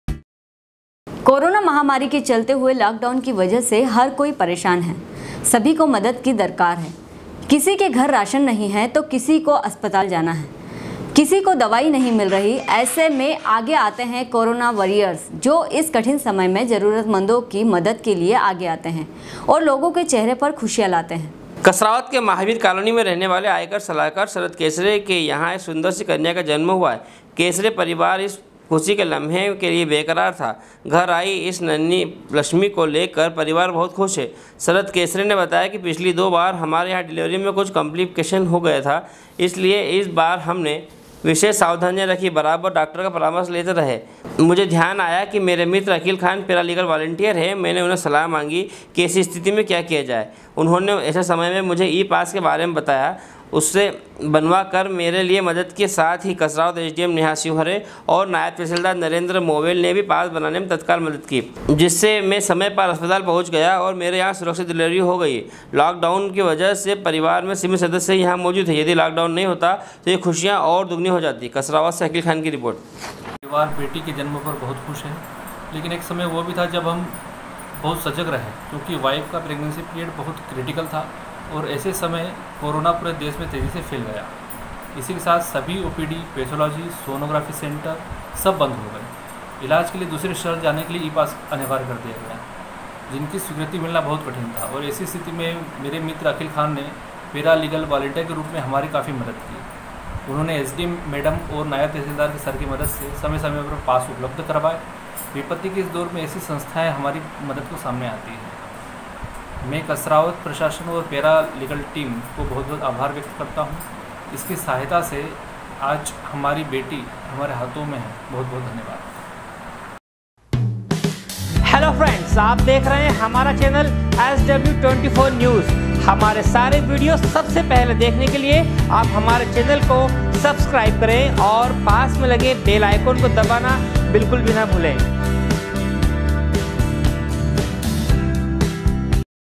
SW 24 News Madhyapradesh#kasrawad#13.05.2020
sw sw24news sw24newsmadhyapradesh livenews news24कोरोना महामारी के चलते हुए लॉक डाउन की वजह से हर कोई परेशान है सभी को मदद की दरकार है किसी के घर राशन नहीं है तो किसी को अस्पताल जाना है किसी को दवाई नहीं मिल रही, ऐसे में आगे आते हैं करोना वारियर्स जो इस कठिन समय में जरूरतमंदों की मदद के लिए आगे आते हैं और लोगों के चेहरे पर खुशियां लाते हैं
कसरावद से अकिल खान कर रिर्पोट